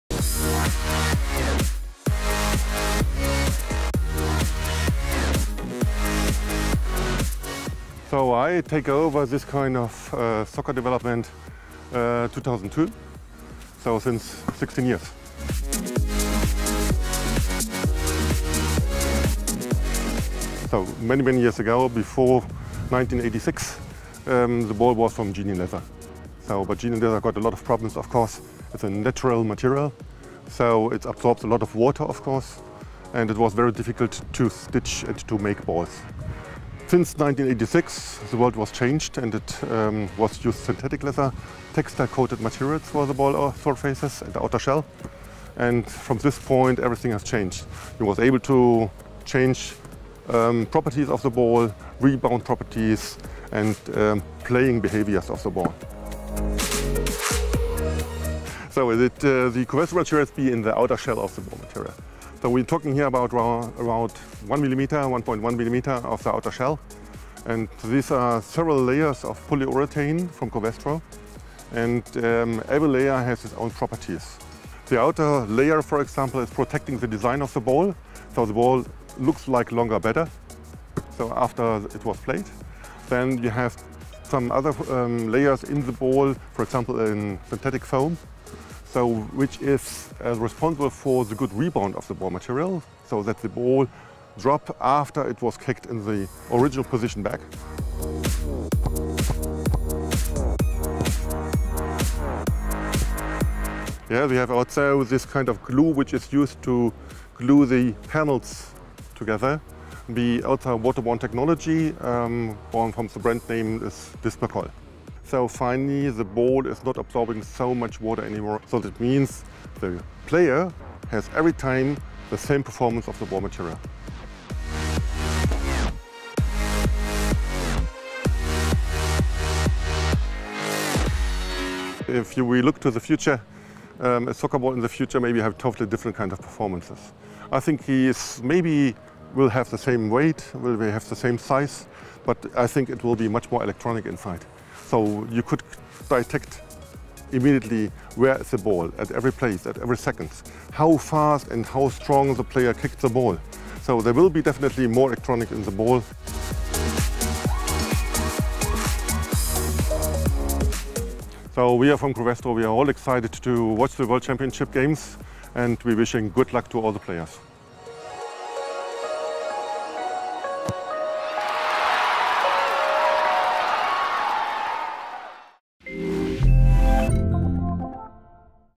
Why Telstar 18 is the most advanced World Cup Match Ball to date
Thomas Michaelis is Head of Covestro Textile Coatings EMLA. Covestro has developed and supplied advanced materials for adidas soccer balls for more than 30 years.